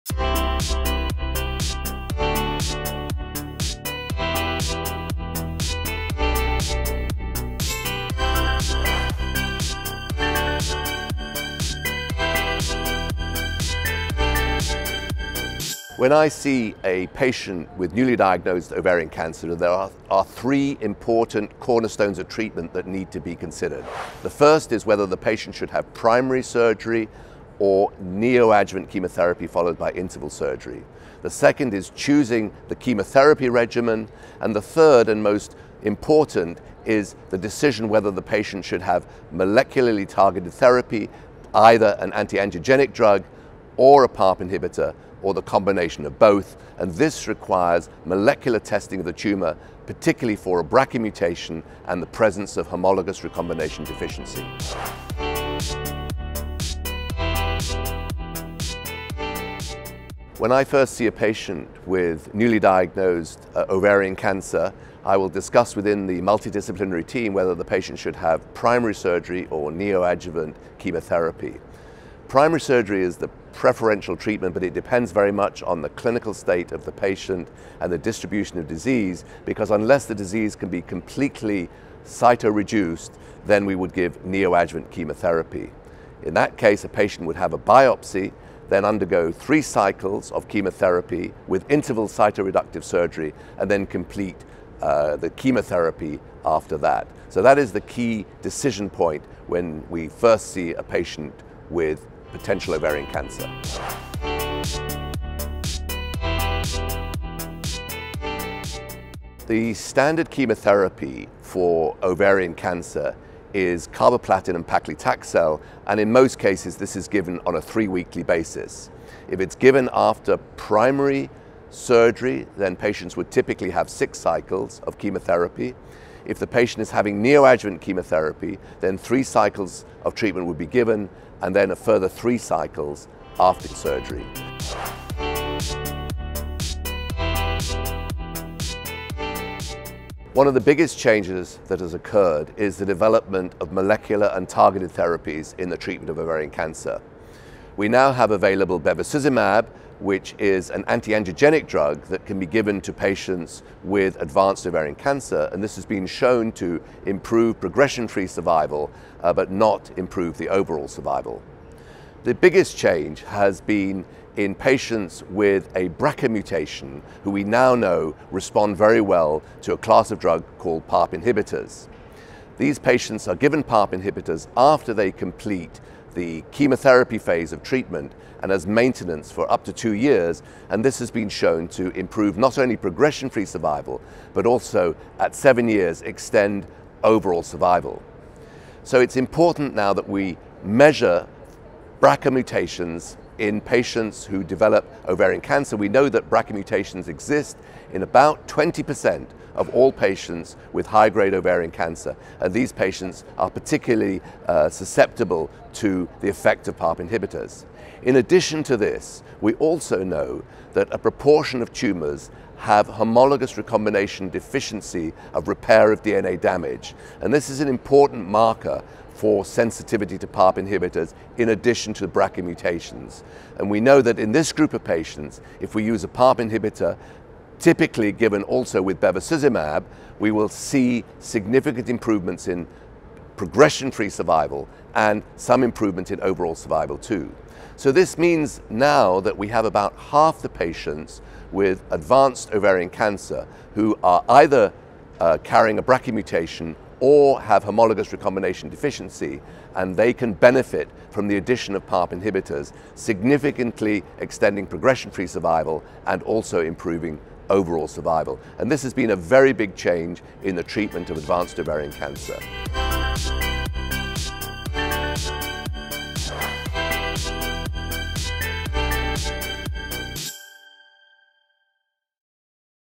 How to determine best first line treatment for ovarian cancer?
Speaker: Jonathan Ledermann, UK
In this ESMO How To video, Jonathan Ledermann advises how to discuss primary surgery vs neoadjuvant chemotherapy with patients with Ovarian Cancer. He explains how to select first-line treatment regimen, and identify patients who may benefit from molecularly targeted therapies.